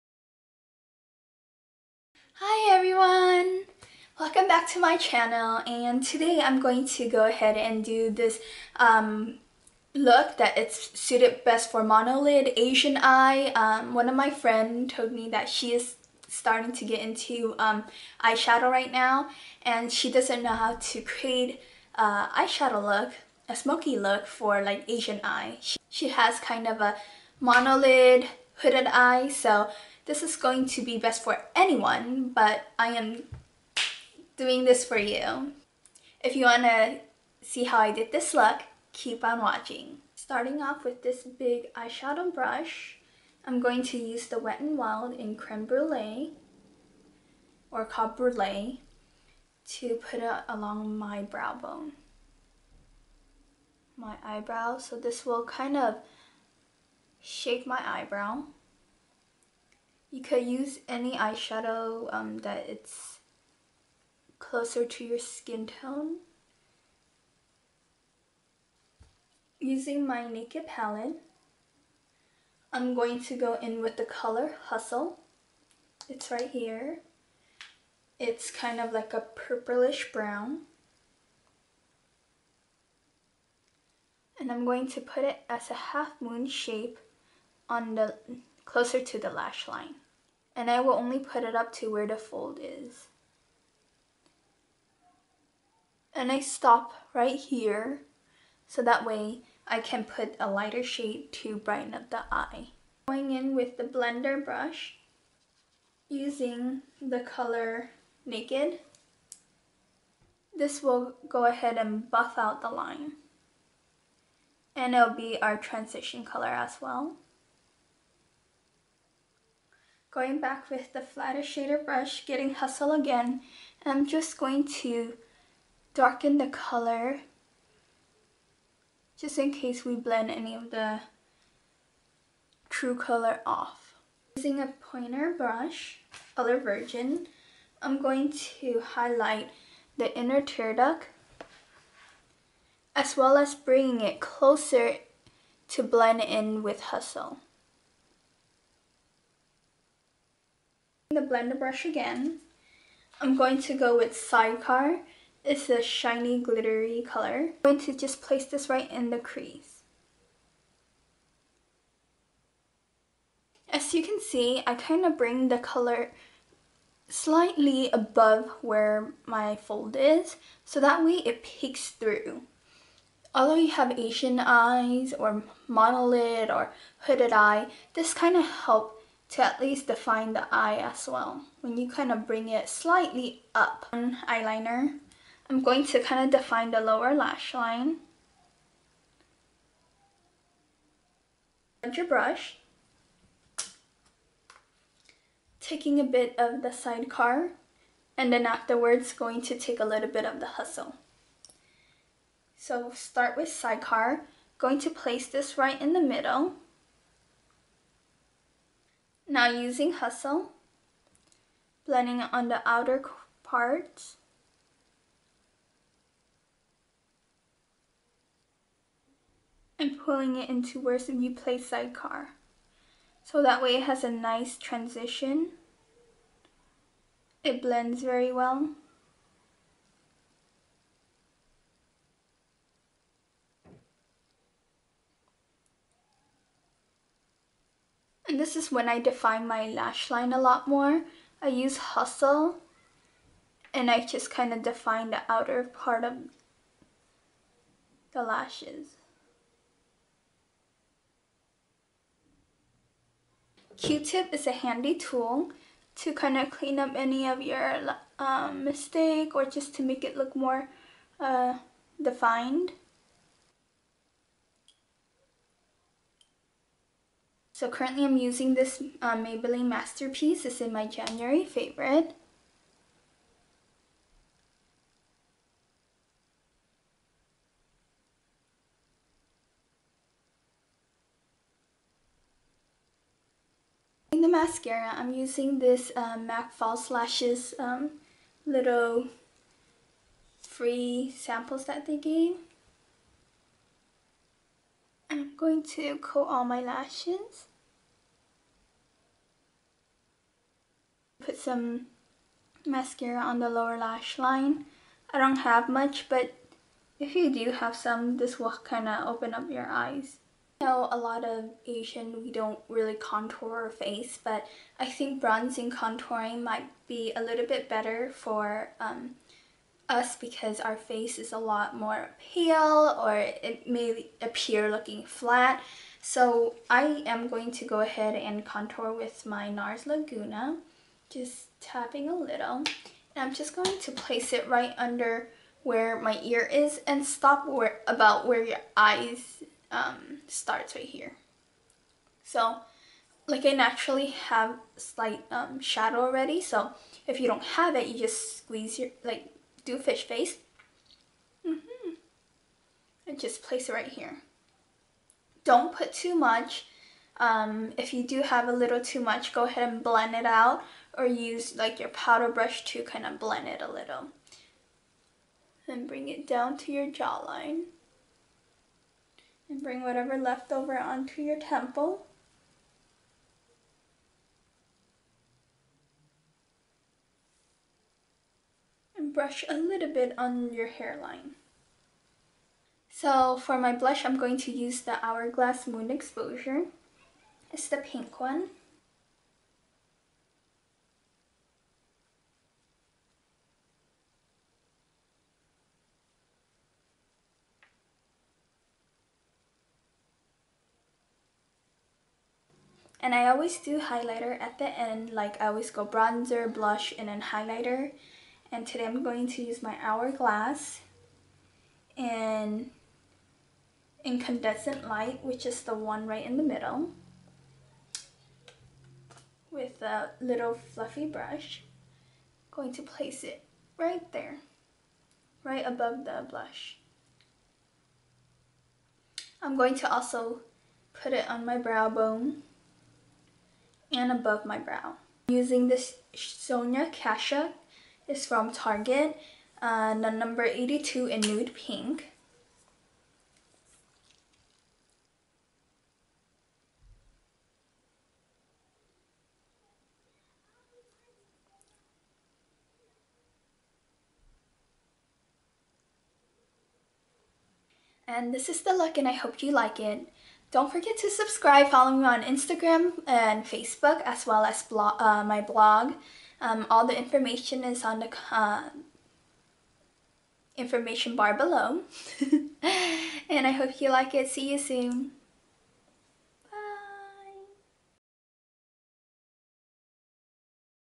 Smokey Eyes for Mono Lid Asian Eyes
Eyes:
Urban Decay Primers
Wet N' Wild in Brulee
Urban Decay Naked Palette - Hustle, Naked, Sidecar
Maybelline Eye Studio Master Precise Ink Pen Eyeliner
M.A.C. Mascara

Face:
Kat Von D Foundation
Korean Powder forgot the Brand name
NARS Bronzer in Laguna
Hourglass Blush Trio
Hourglass Highlighter Trio

Lips:
Sonia Kashuk Satin Luxe Lip Color in 82 Nude Pink

I have created another Smokey Eyes Look for Asian eyes for a friend who recently told me she is starting to play with some eye shadows but unsure how to do it for Asian shaped eyes. My friend, I hope this help.

Hope this help and if you want to get more information on some of what I am liking check out my blog.